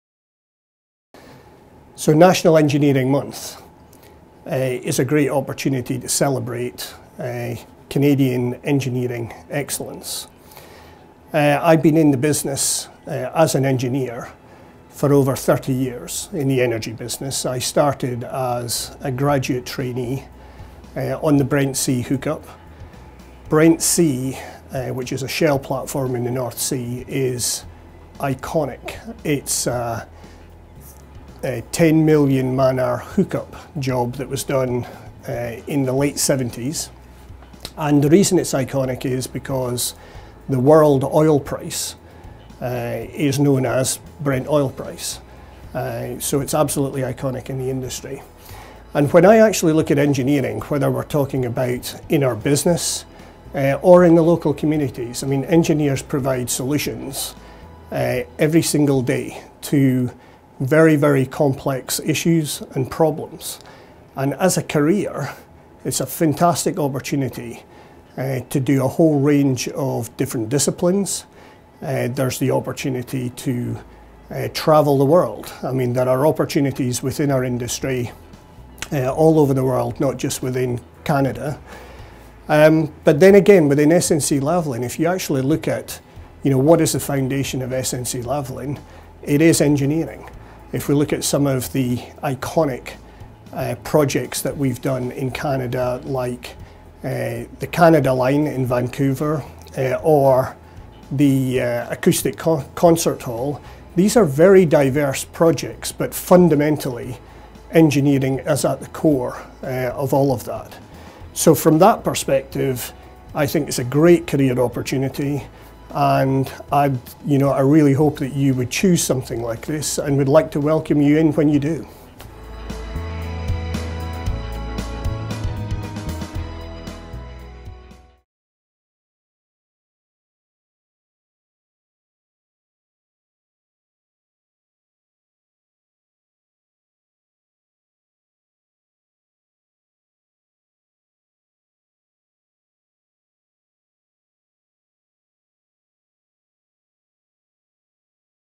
Neil Bruce discussing National Engineering Month
Neil Bruce, President Resources and Environment & Water sharing his thoughts on National Engineering Month.